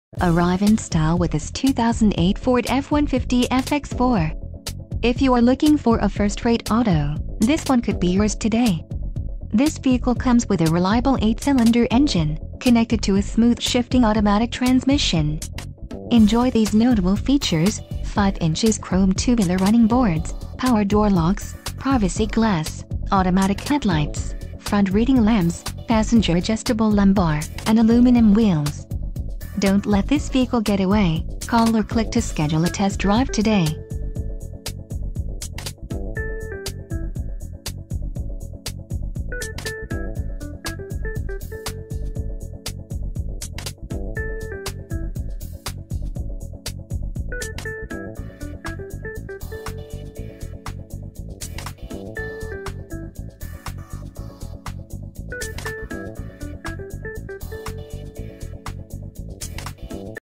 2008 Ford F-150 FX4 - Phil Long Ford of Chapel Hills - Co...
Superb Condition. Bright Red with Dark Shadow Grey Metallic exterior and Black interior, FX4 trim. Heated Seats, iPod/MP3 Input, CD Player, Fourth Passenger Door, 4x4, 20" 6-SPOKE MACHINED ALUMINUM WHEELS, LUXURY PKG, Aluminum Wheels, 5" CHROME TUBULAR RUNNING BOARDS, Consumer Guide Recommended Pickup, JDPower.com - 4 Power Circle Rated, Edmunds.com's review says "Smooth and quiet ride.", Non-Smoker vehicle, Locally Owned. 5 Star Driver Front Crash Rating. SEE MORE!KEY FEATURES INCLUDE4x4, iPod/MP3 Input, CD Player, Fourth Passenger Door. MP3 Player, Third Passenger Door, Privacy Glass, Keyless Entry, Child Safety Locks. OPTION PACKAGESLUXURY PKG: heated driver & front passenger w/drivers seat memory, pwr front passenger seat w/manual lumbar, electronic auto temp control, steering wheel mounted audio & climate controls, pwr adjustable pedals w/memory, heated exterior mirrors w/mounted turn si... read more